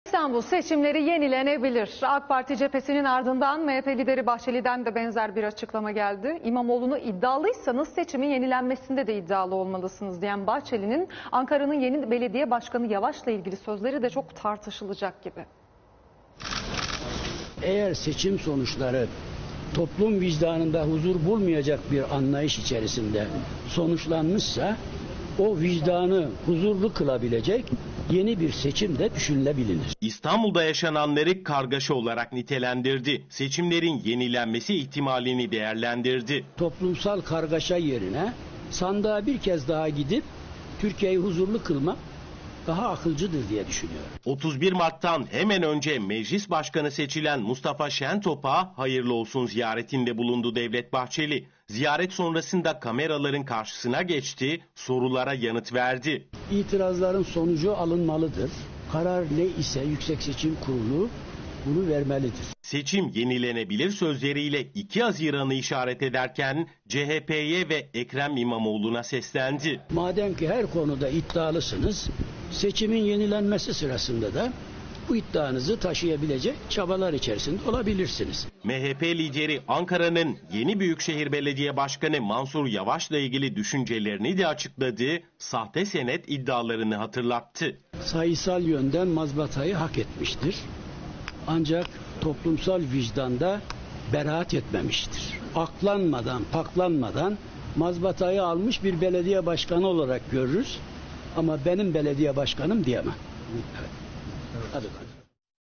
Bahçeli: "Yeni bir seçim düşünülebilir"
İSTANBUL SEÇİMLERİ YENİLENEBİLİR. AK PARTİ CEPHESİNİN ARDINDAN MHP LİDERİ BAHÇELİ'DEN DE BENZER AÇIKLAMA GELDİ.. İMAMOĞLU'NA "İDDİALIYSANIZ, SEÇİMİN YENİLENMESİNDE DE İDDİALI OLMALISINIZ" DİYEN BAHÇELİ'NİN , ANKARA'NIN YENİ BELEDİYE BAŞKANI YAVAŞ İLE İLGİLİ SÖZLERİ DE
ÇOK TARTIŞILACAK GİBİ.